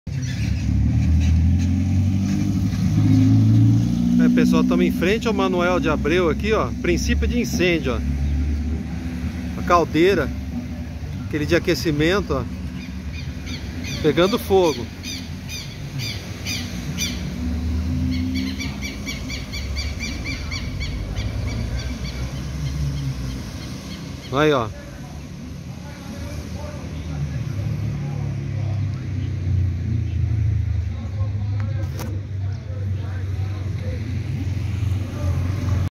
Bauru: Hospital Manoel de Abreu tem princípio de incêndio
Crédito: Marivaldo do Drone